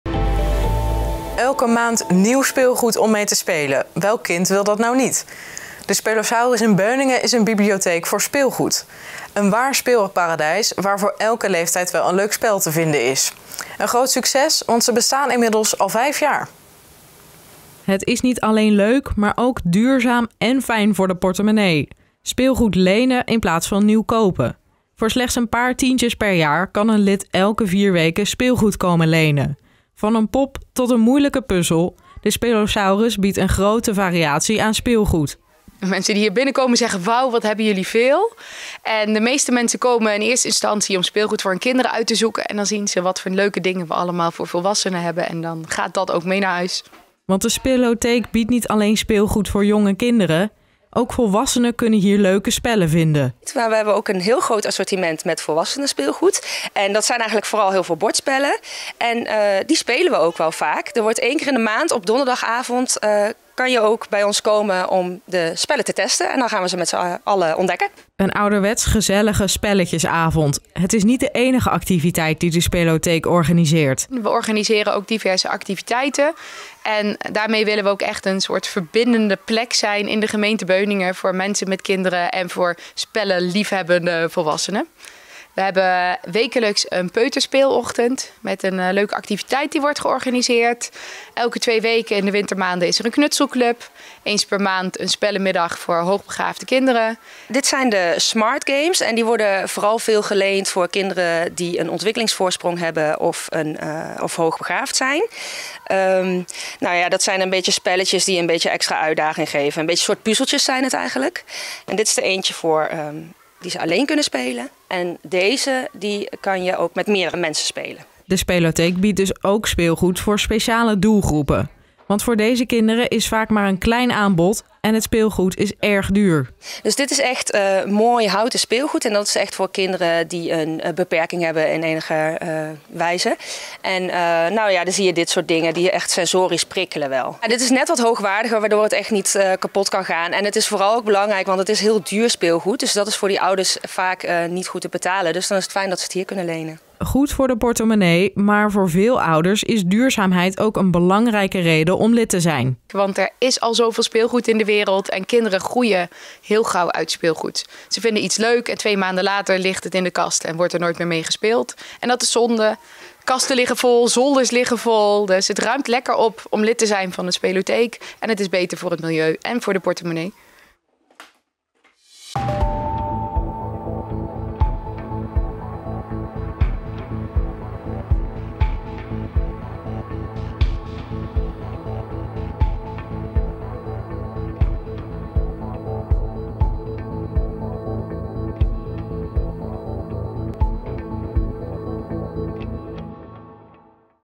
Speelotheek Beuningen al vijf jaar een succes || RN7 REGIONIEUWS
Elke maand nieuw speelgoed om mee te spelen, welk kind wil dat nou niet? De Speelosaurus in Beuningen is een bibliotheek voor speelgoed. Een waar speelparadijs, waar voor elke leeftijd wel een leuk spel te vinden is. Een groot succes, want ze bestaan inmiddels al vijf jaar.